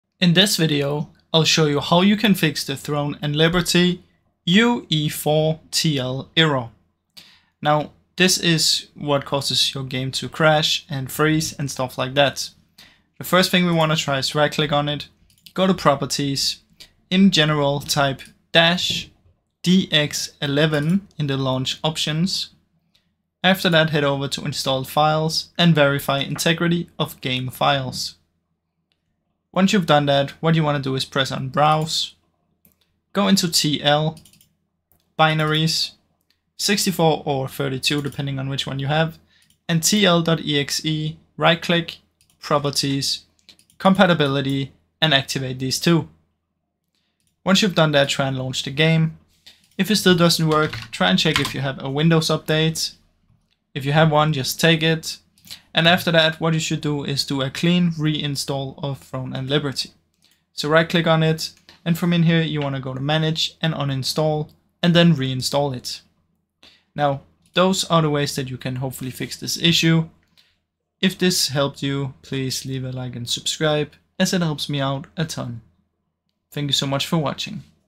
How To Fix Throne & Liberty Crashing (UE4 TL Error) - Full Tutorial
In this video I'll show you how to easy and fast fix UE4 TL Error in Throne And Liberty. The method is very simple and clearly described in the video. Follow all of the steps & see how you can fix crashing and freezing issues in Throne & Liberty